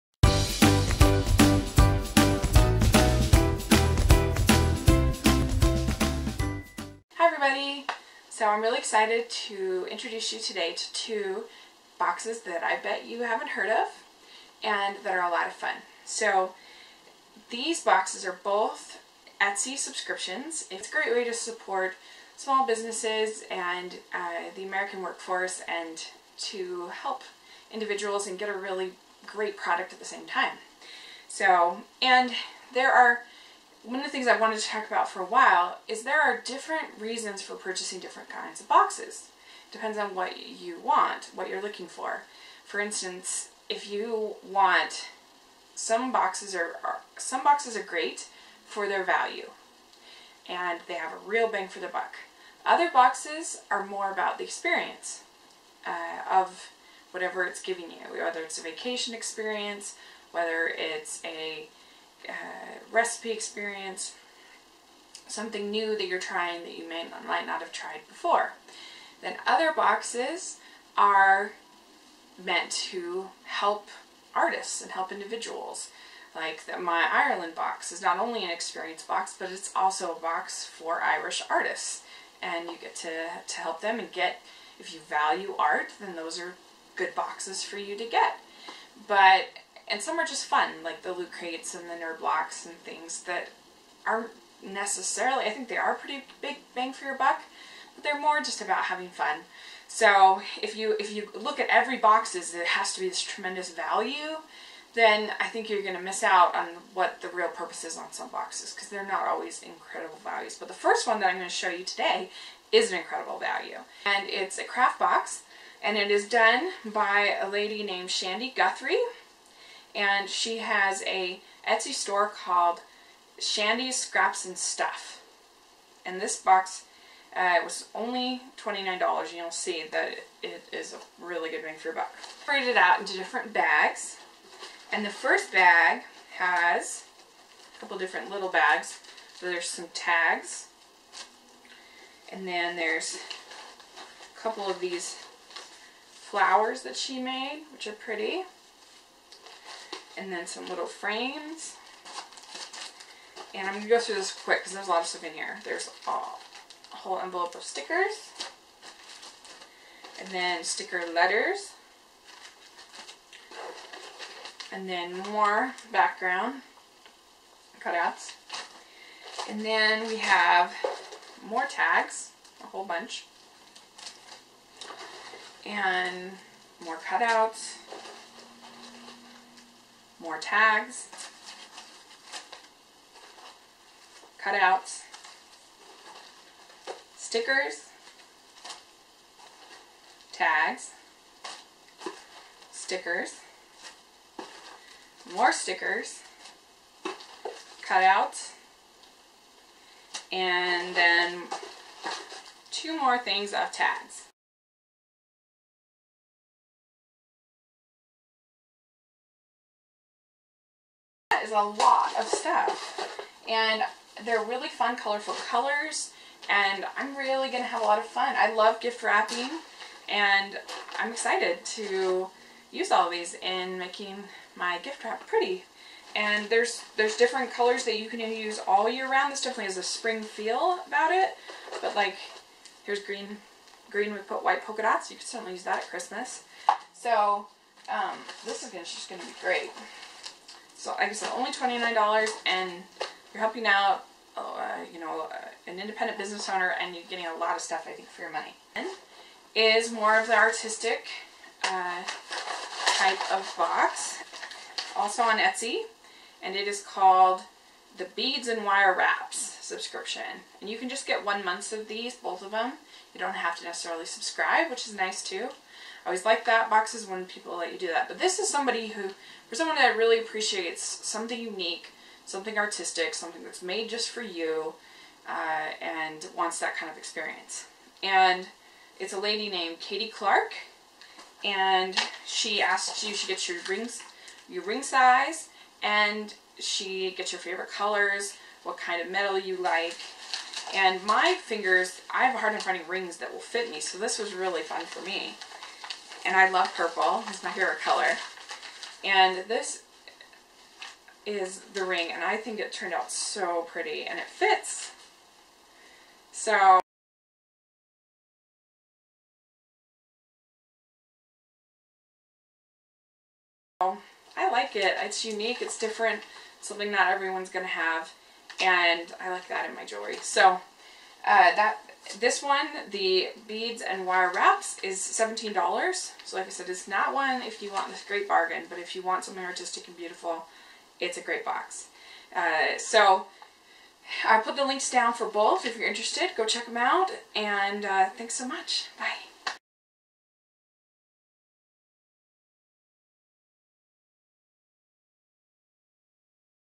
Two Etsy Subscriptions
2 fun etsy boxes today.  I also talk a little bit about why different boxes are for different needs.  Not all are huge bang for your buck but offer other benefits,